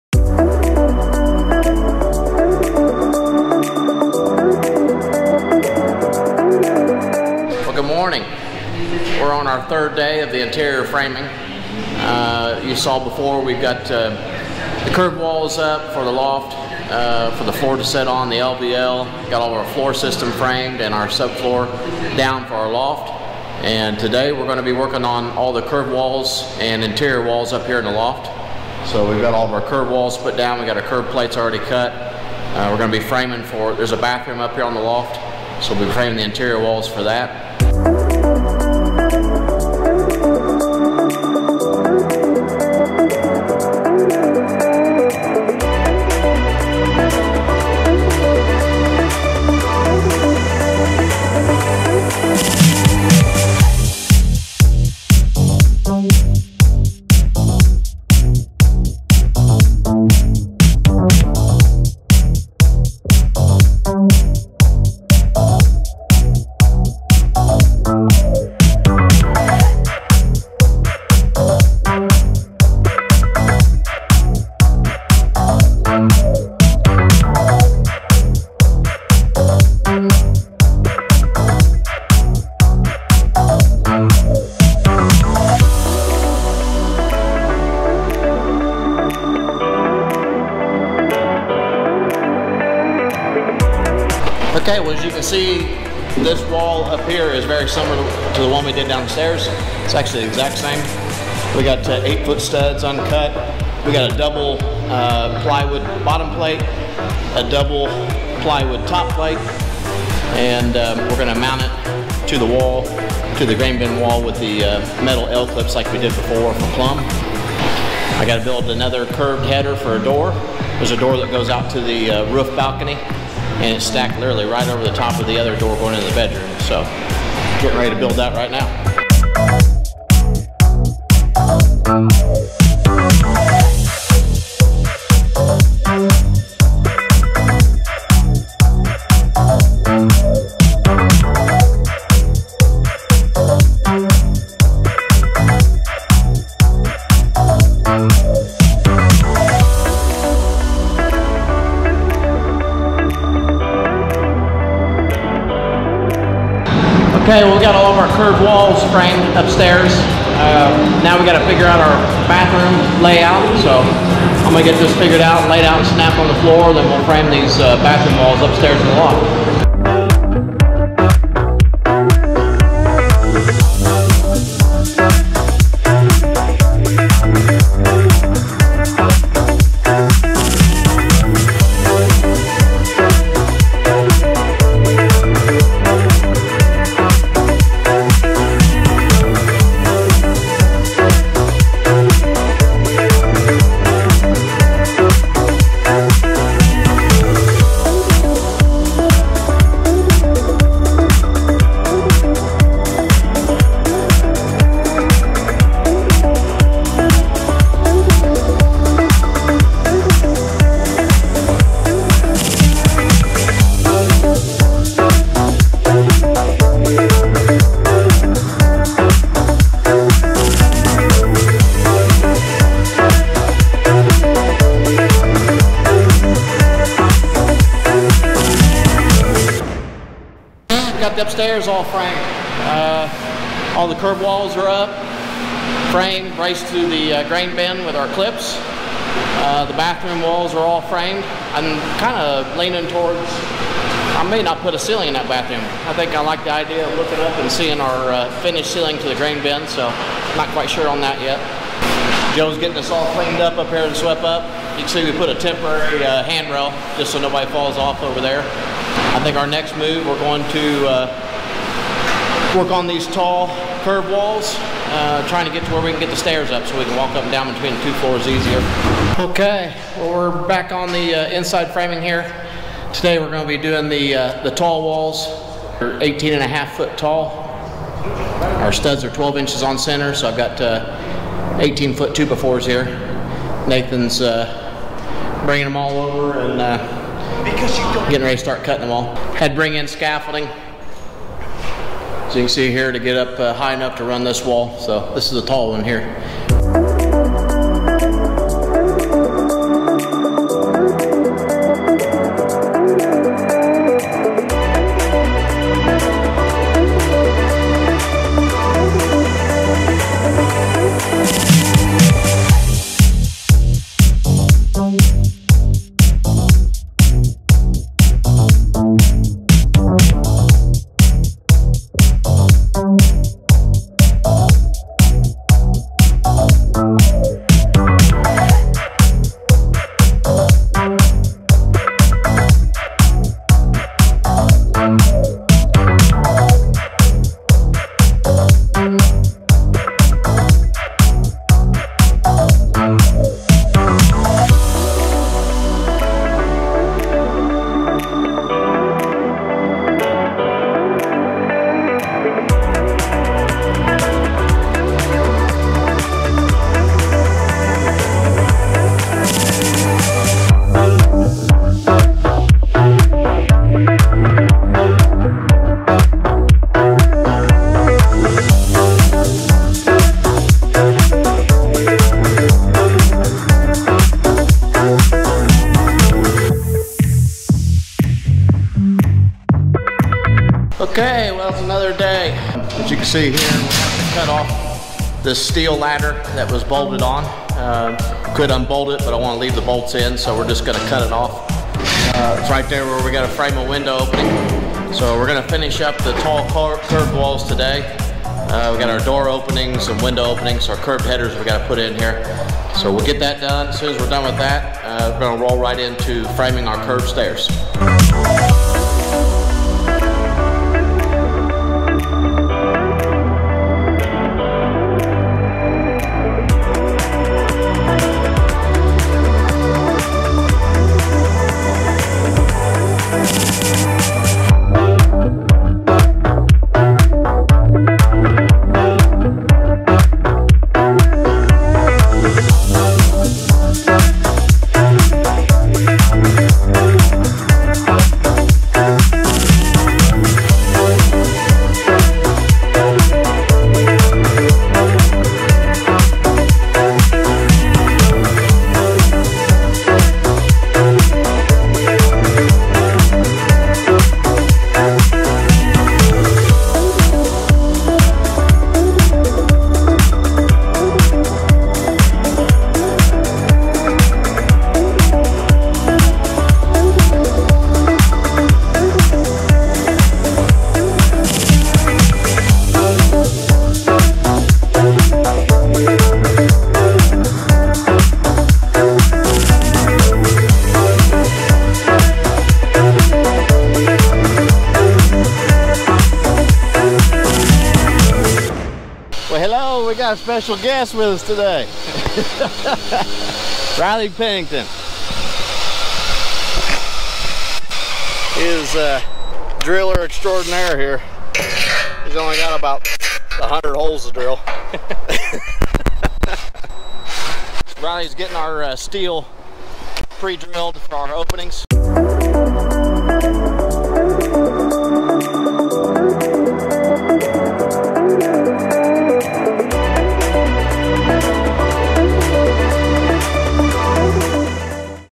Grain Bin Home Build... Episode 11 "Interior framing part 3"
In this 11th episode, we are continuing the interior framing of the Grain Bin.  Curved exterior walls in loft, and interior bathroom walls for loft.  Tall curved exterior walls, curved headers for window and door openings.  Grain Bin Home, Grain Bin Remodel, Grain Bin Conversion, Grain Silo Home, Grain Bin House, Tiny House.